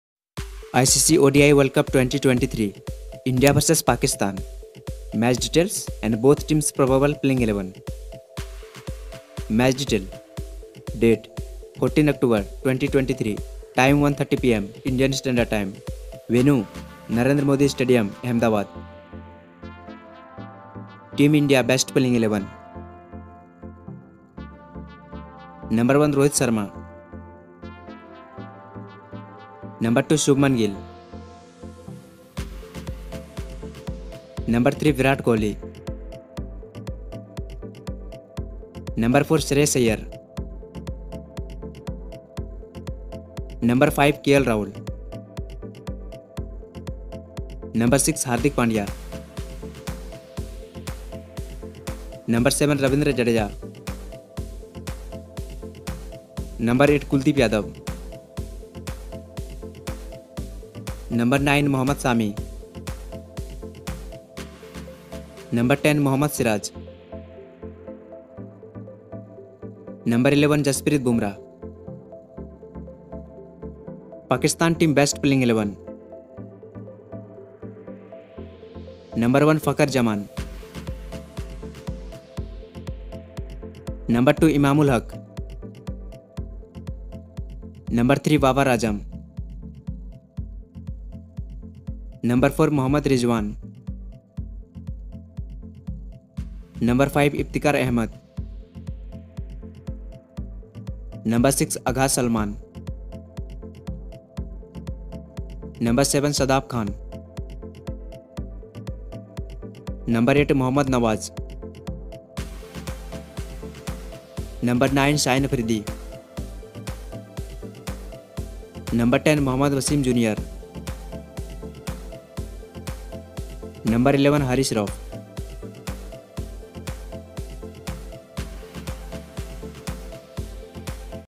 ICC World Cup 2023 India vs Pakistan Playing 11 | Ind vs Pak Match Playing 11 Ind vs Pak 2023
Icc Cricket World Cup 2023 | India vs Pakistan Match Detail & Playing 11

Match Detail :-

Time - 1:30 Pm Indian Standard Time
Venue - Narendra Modi Stadium Ahmedabad

India Team Playing 11

(C) Rohit Sharma, Shubman Gill, Virat kohli, Shreyas Iyer, (WK) Kl Rahul,Hardik Pandya, Ravindra Jadeja, Kuldeep Yadav, Shardul Thakur , Jasprit Bumrah, Mohammad Siraj

Pakistan Team Playing 11

Fakar Zaman, Imamul Haqe  , (C) Babar Azam, (WK) Mohammad Rizwan, Iftikhar Ahmed  Agha Salman, Shadab Khan, Mohammad Nawaz, Shaheen Afridi, Harish Rauf, N
Mohammad Wasim Junior

All The Images And Pictures Ind The Video Showes Belongs Respected Owner And Not Me